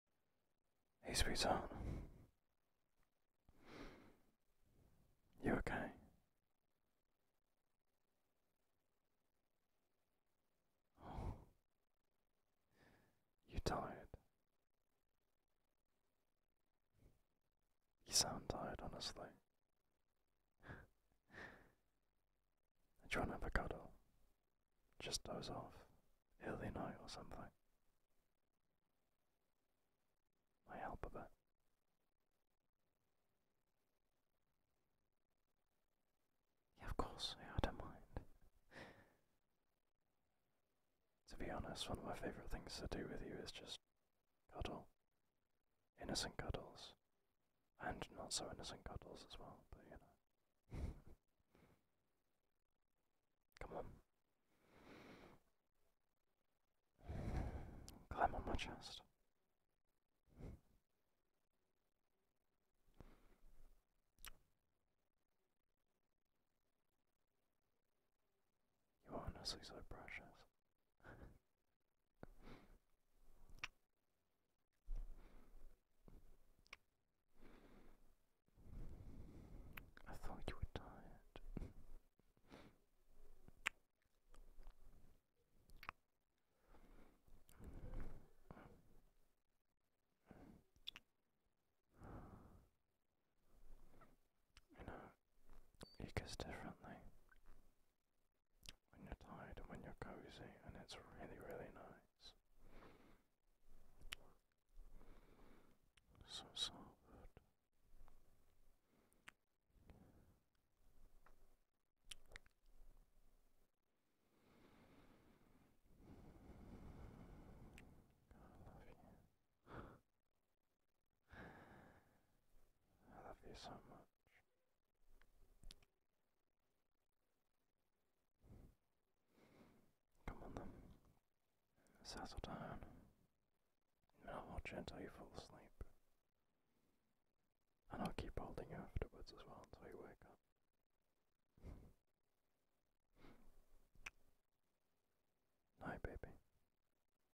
POV Your Boyfriend Holds You Close [Spicy] ASMR [M4A] [Cute] [Kissing] [Cuddling] [Cosy]
After realising that you're sleepy, your boyfriend invites you to snuggle up...

A massive thank you to my members! (All channel members will have their name displayed here :): Rin, 艾維Avie, Letty